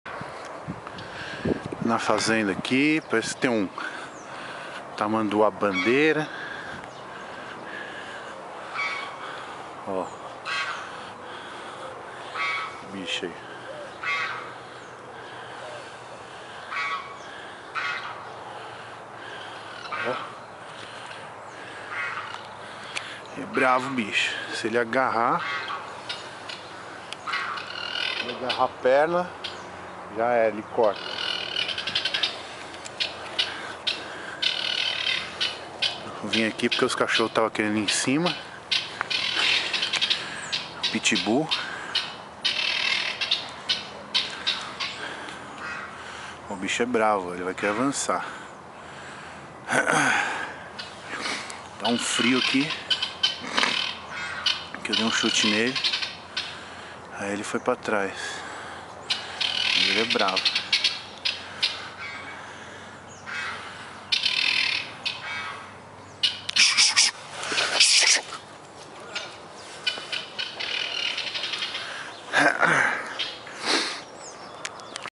tamandua bandeira fez uma visita na fazenda
dia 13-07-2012 tamandua fez uma visita na fazenda.. brigo com meu dog.. e deu trabalho hehe